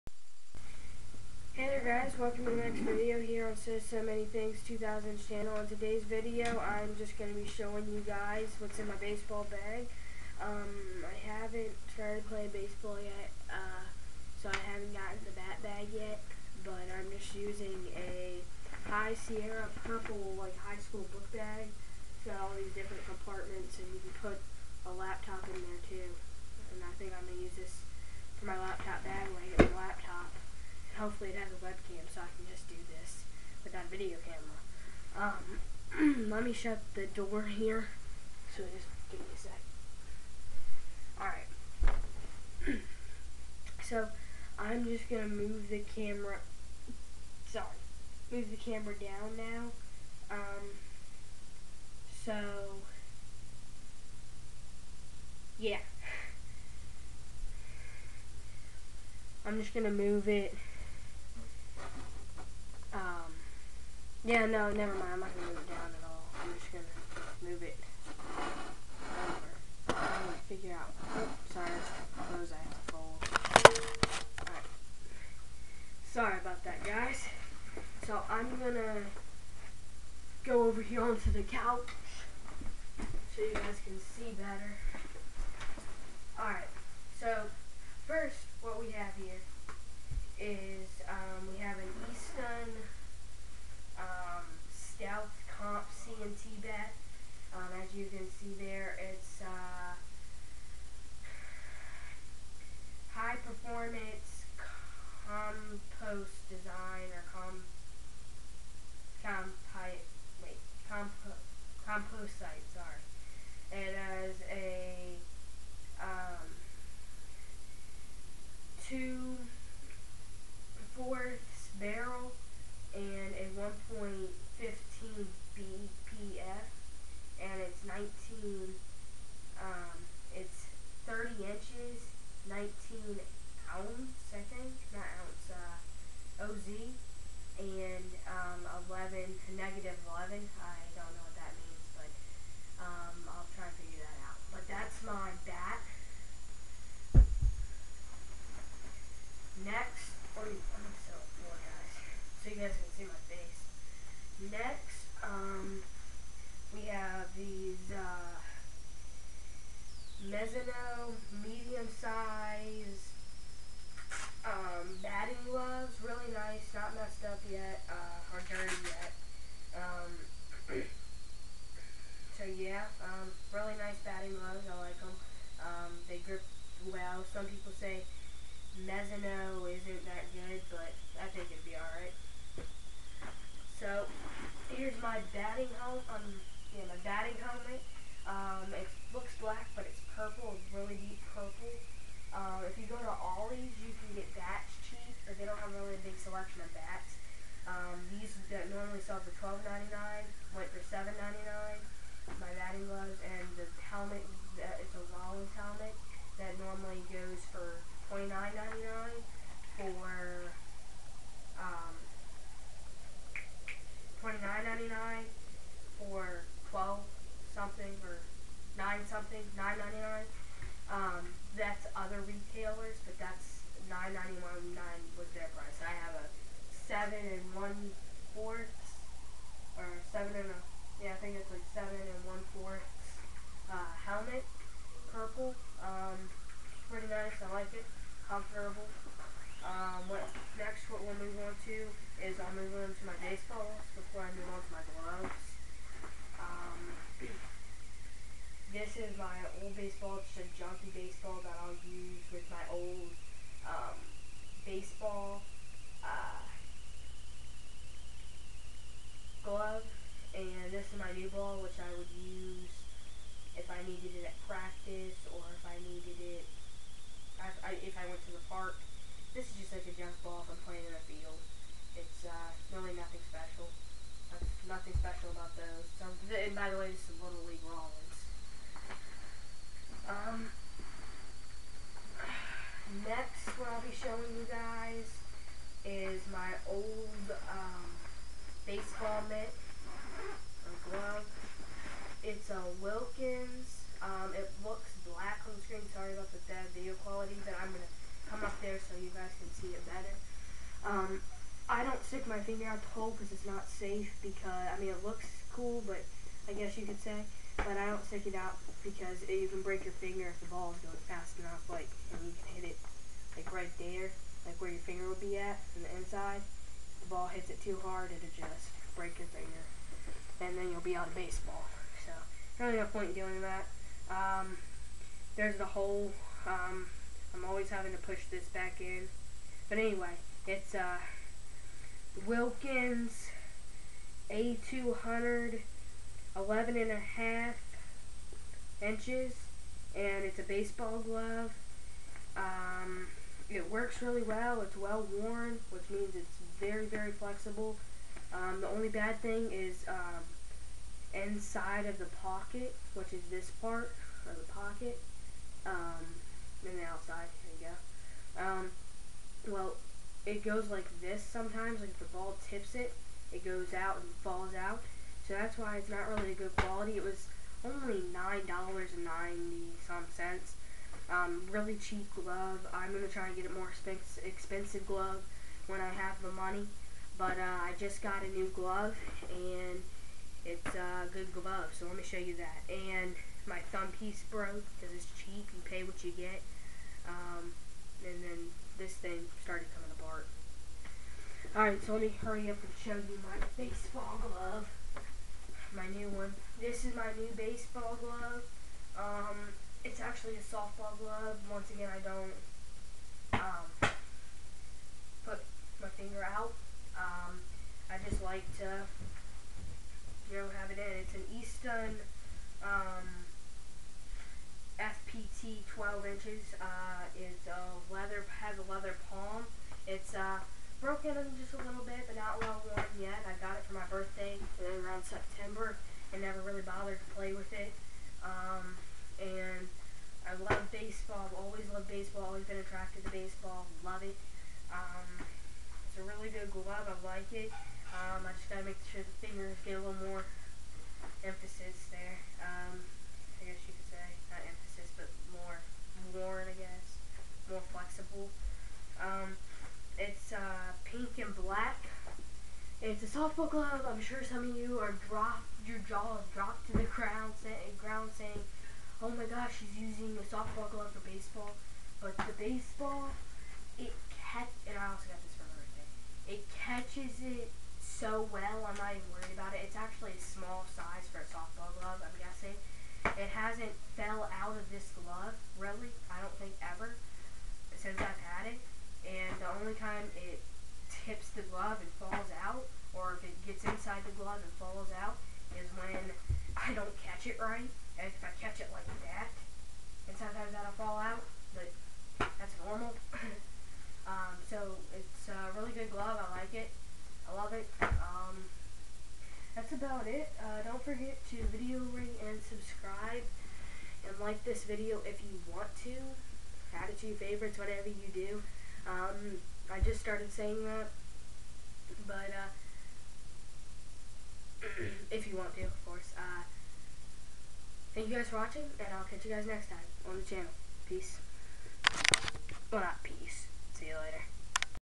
whats in my bat bag
this is just whats in my bat bag and i should be starting baseball soon and getting a bat bag soon i also am going to play spring ball for little league and hope to have many more seasons